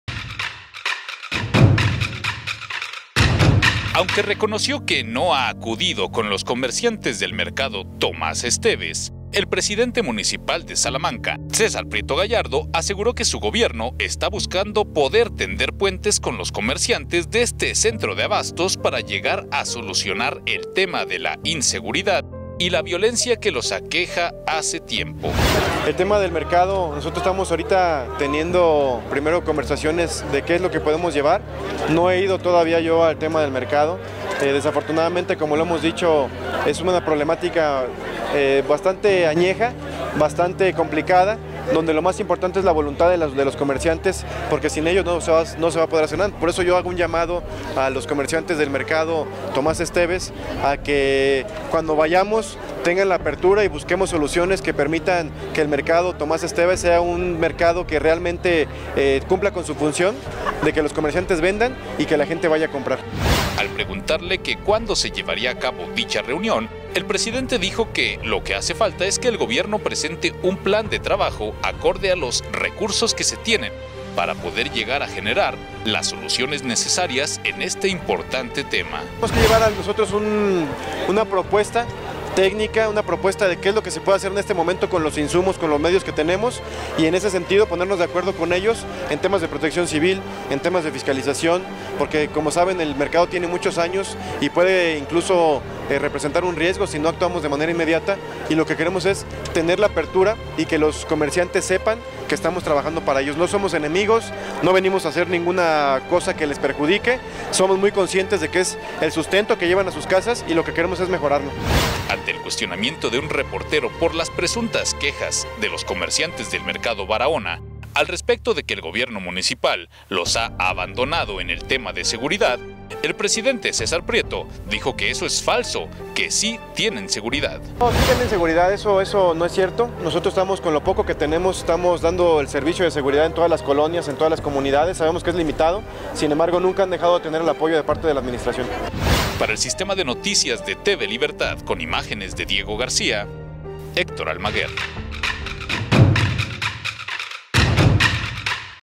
Busca el Gobierno de Salamanca ponerse de acuerdo con los comerciantes de los mercados
Busca el Gobierno de Salamanca ponerse de acuerdo con los comerciantes de los mercados Tomasa Esteves y Barahona, asegura que sí tienen seguridad.